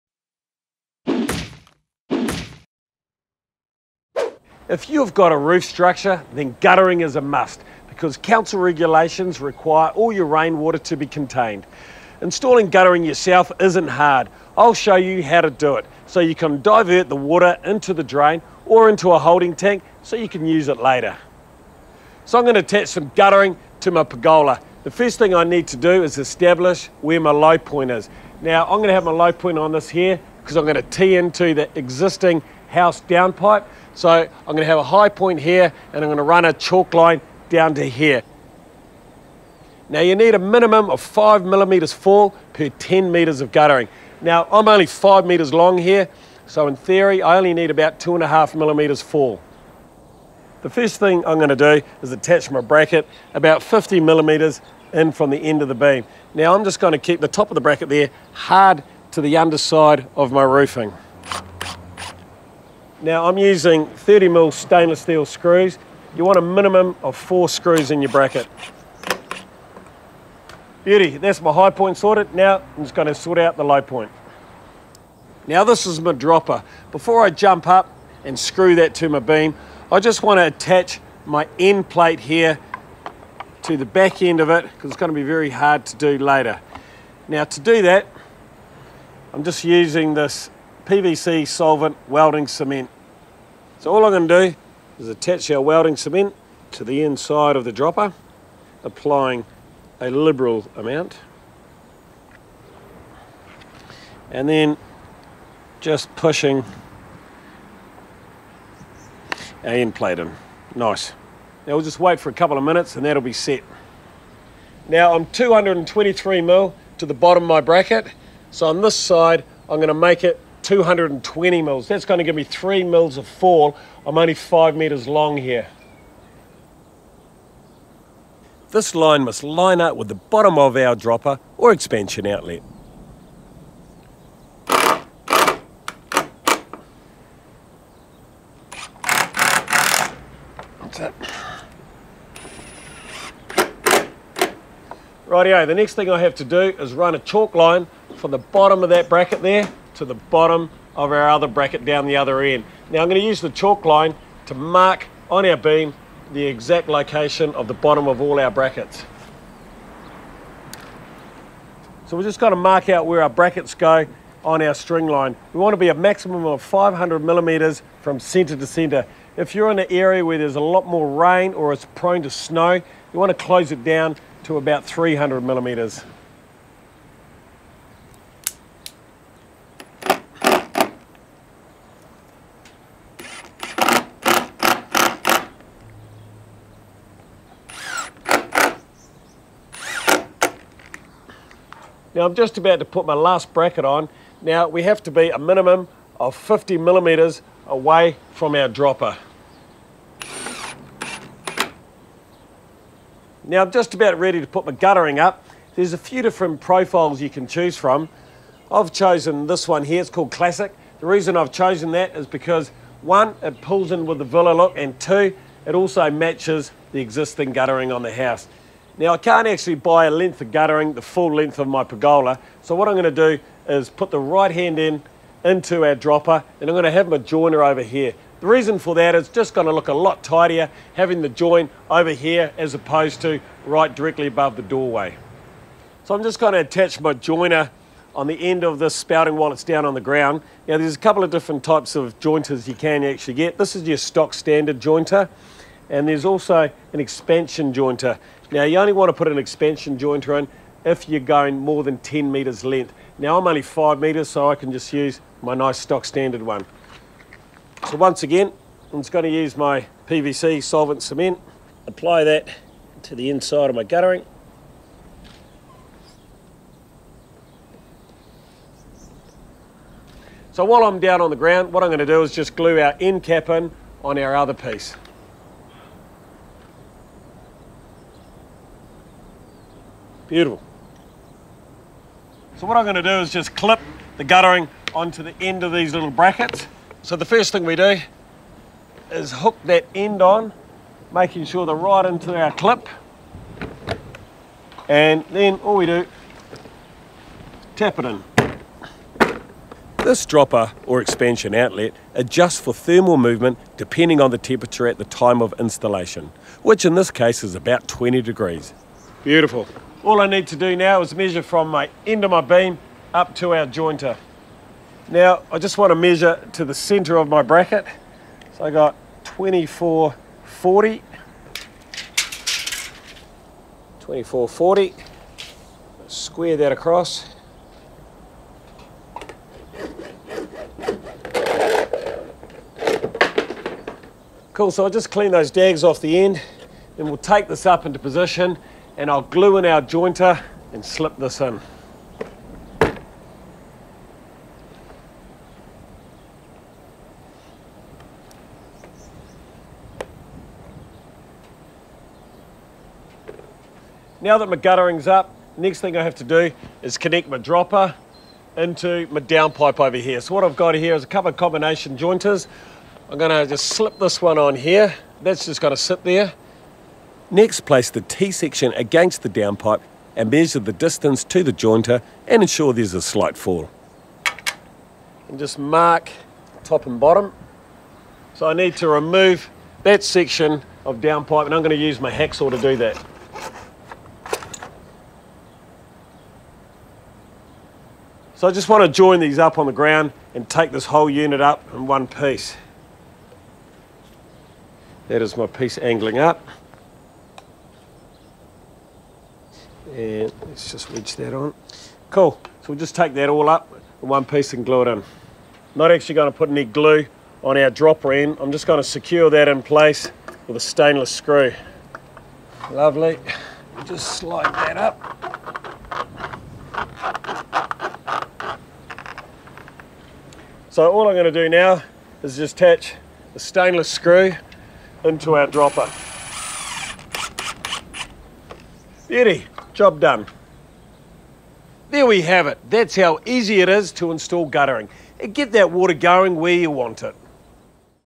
How to Install Guttering | Mitre 10 Easy As DIY
If you have a roof structure then guttering is a must because council regulations require all your rainwater to be contained. Installing guttering yourself isn’t hard, I’ll show you how to do it with Mitre 10.